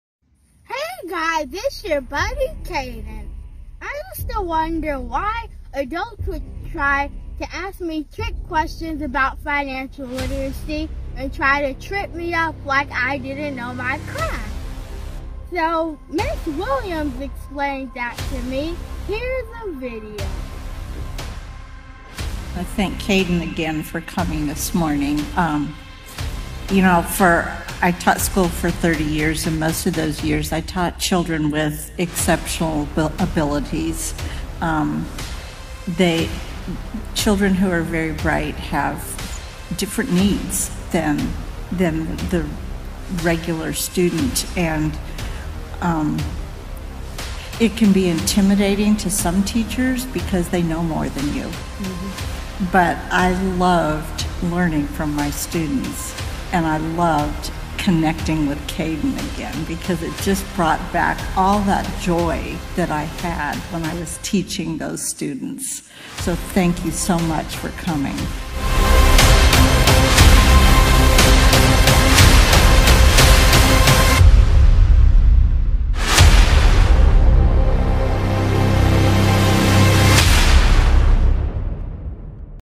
Dr Sean J Harris discusses why adults download kids scholar and genius
Free Magazine:
GWBlueprint.com